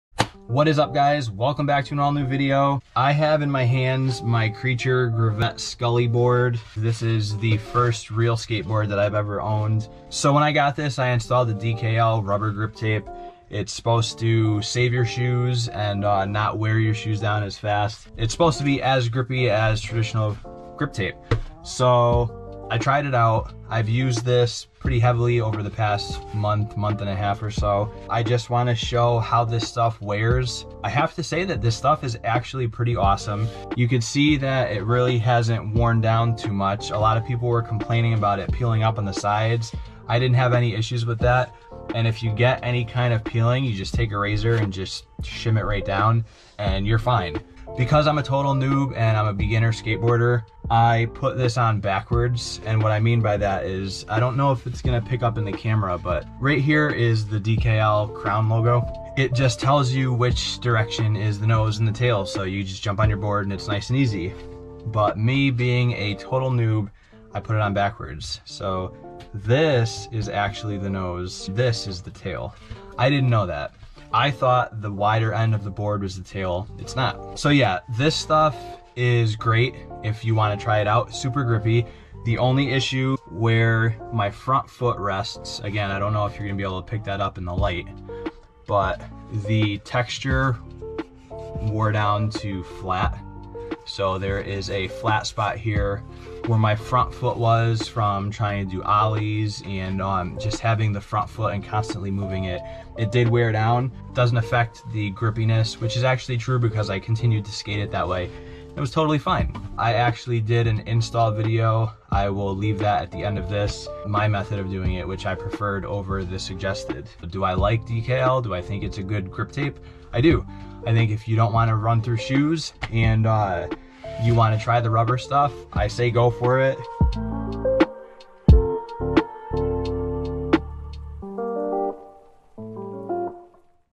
SKATING DKL RUBBER GRIP FOR A MONTH!!! DKL Rubber Grip Tape
What's up guys, and welcome back to an all new video.  It's been a few months since I started skateboarding, and one of the first things I did when I got my Creature complete was install DKL's rubber grip tape.  How does it hold up to a month of abuse?  Let's find out!  Being a beginner, this board got skated hard.  I skated just about every day, just pushing around for hours.  I also got my feet wet with learning some beginner tricks.  I also have a lot that I haven't filmed yet.  I'll say I was very impressed with how well this stuff held up, and how great it felt to skate.  I have a part two to this, where I strip down the board and try out Amgrip grip tape.  That video should be up within the next week.